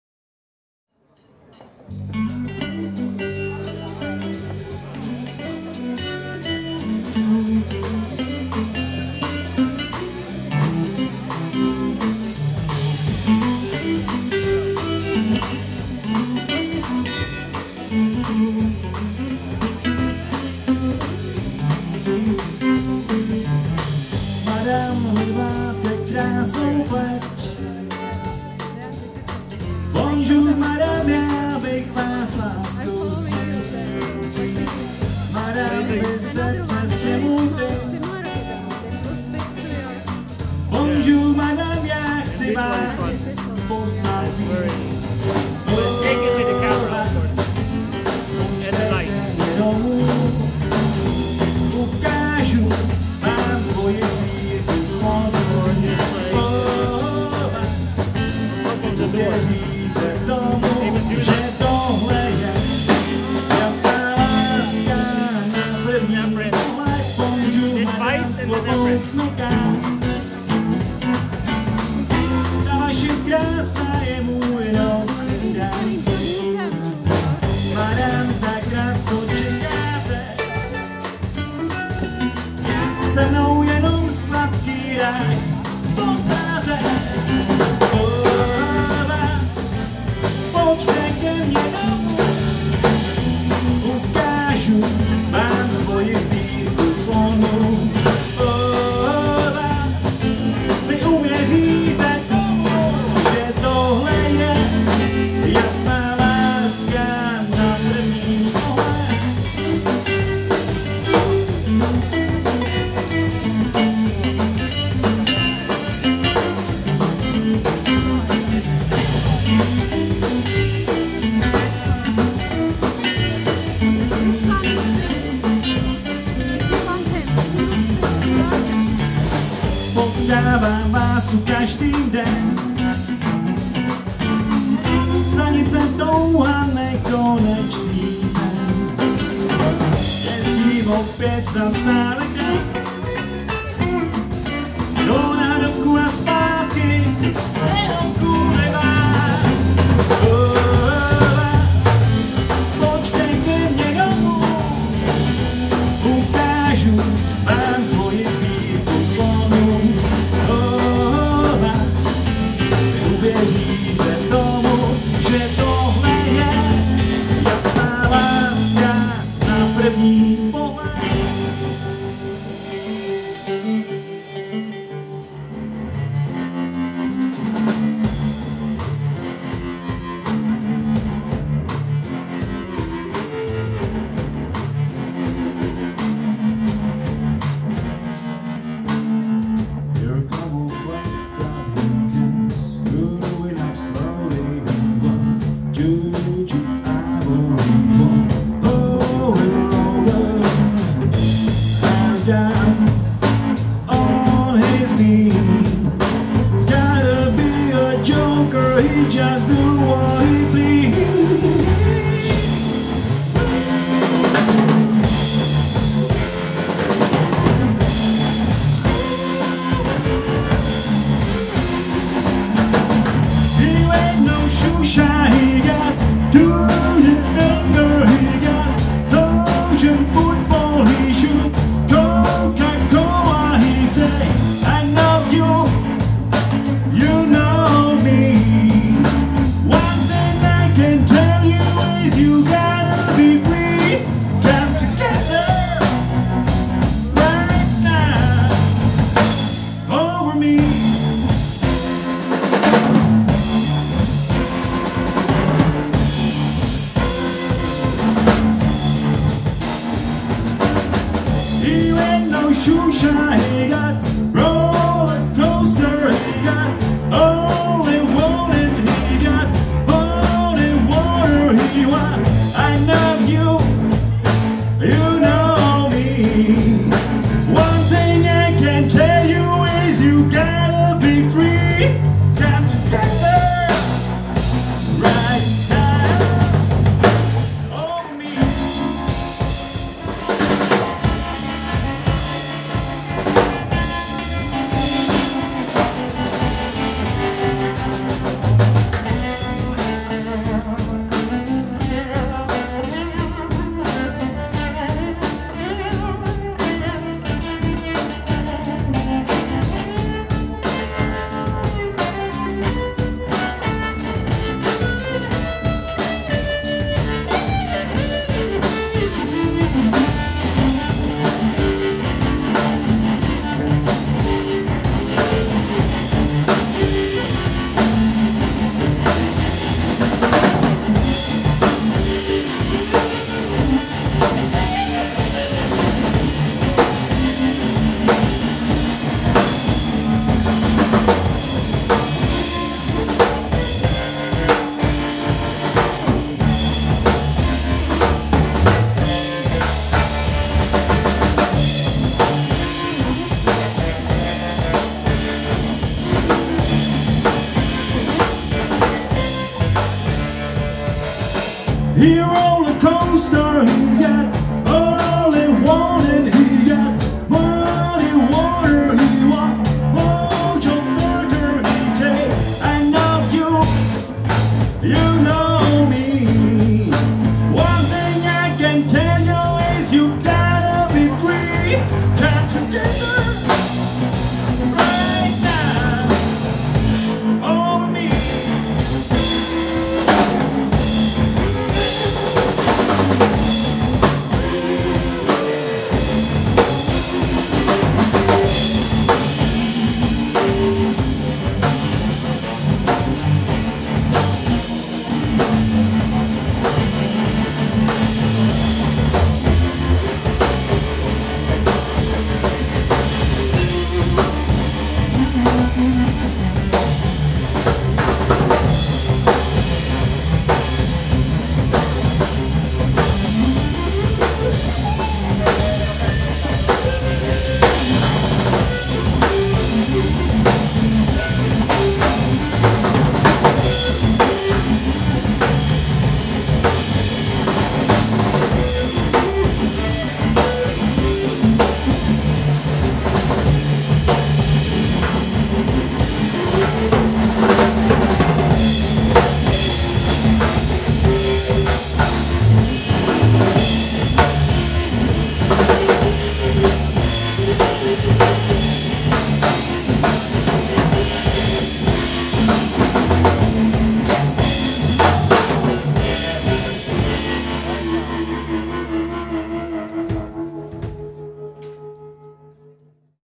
SPINAVY LUZRI - Live Burzu 4
SPINAVY LUZRI live at Burzu 4 in Pražské Tržnici on Friday September 24th 2020